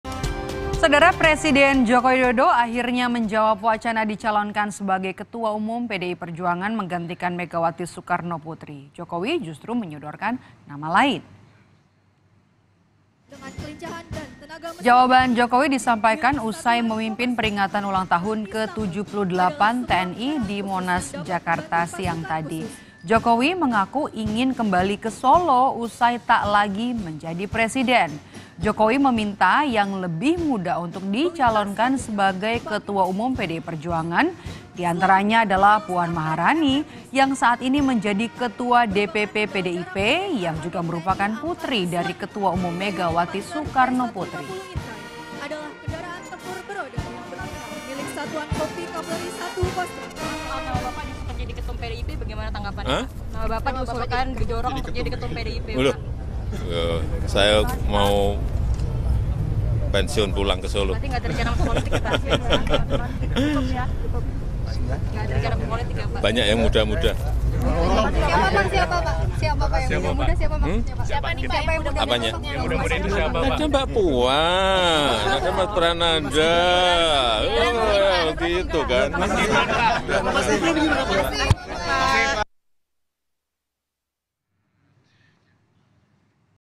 Jokowi Buka Suara soal Wacana Jadi Ketum PDIP: Saya Pensiun Mau ke Solo
KOMPAS.TV - Jokowi Buka Suara soal Wacana Jadi Ketum PDIP: Banyak yang Muda

Kompas TV
Independen | Terpercaya